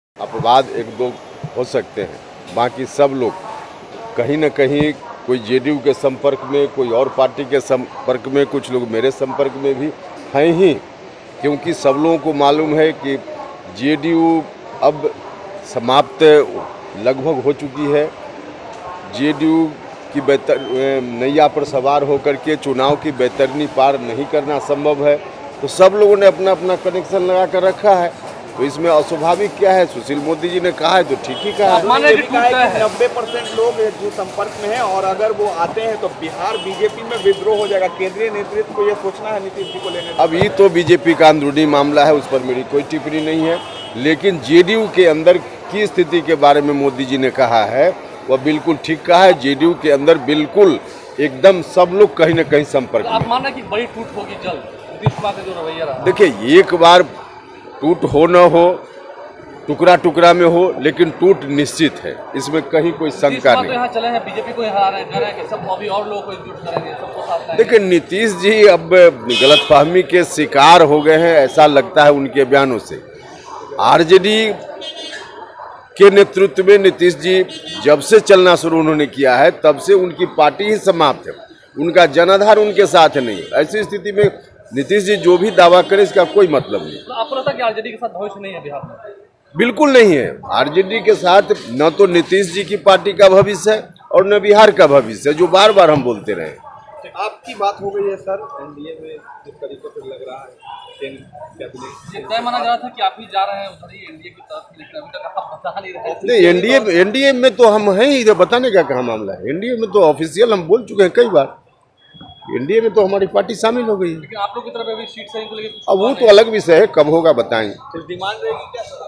Bihar News : 'JDU की नैया पर सवार होकर चुनावी वैतरणी पार करना संभव नहीं' - Upendra Kushwaha
लोकसभा चुनाव से पहले बिहार की राजनीति गरमाई हुई है. एनडीए के नेता महागठबंधन सरकार को लेकर तरह-तरह के दावे कर रहे हैं तो वहीं सत्ता पक्ष तमाम दावों को खारिज कर रही है. सुशील मोदी ने जेडीयू में भारी टूट को लेकर बयान दिया था. अब उपेंद्र कुशवाहा ने भी इस बात का समर्थन करते हुए कहा कि जेडीयू के कई नेता हम लोगों के संपर्क में हैं.